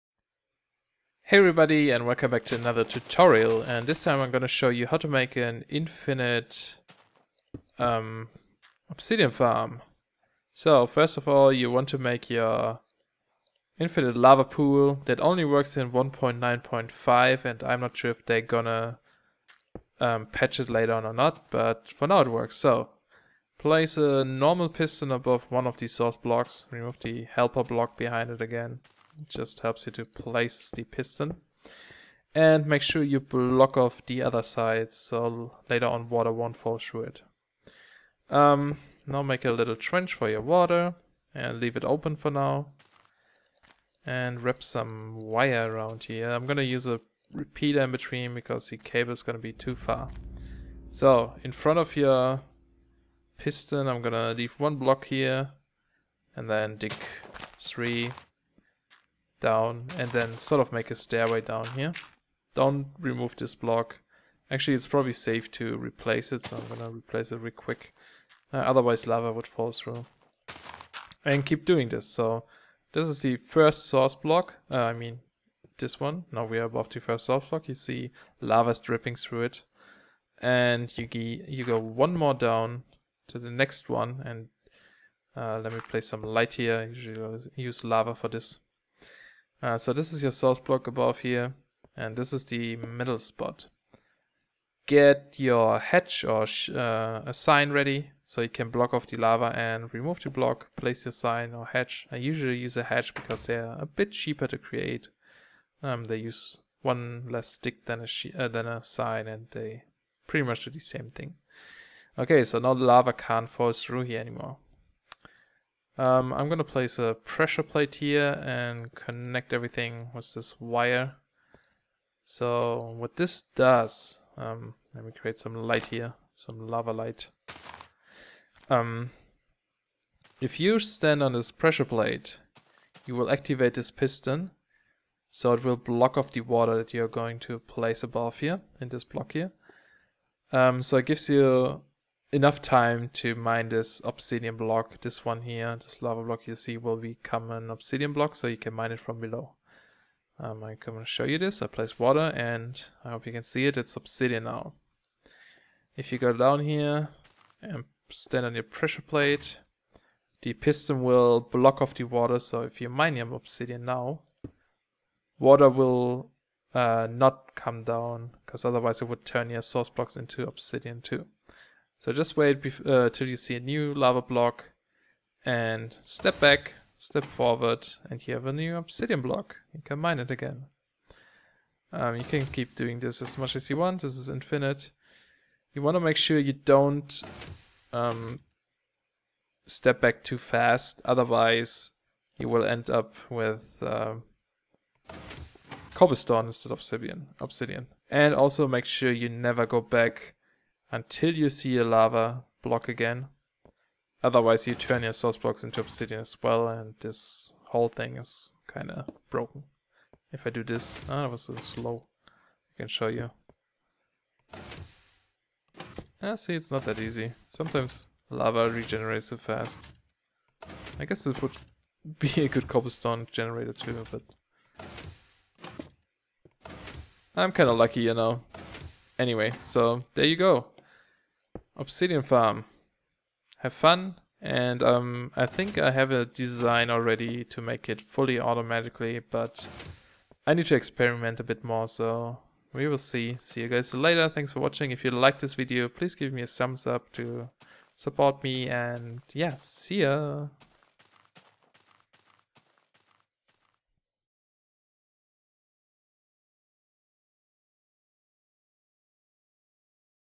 Serious Tutorial - How to make infinite Obsidian (1.9.5)
This is a small tutorial based on Avidya's tutorial for "How to make infinite lava"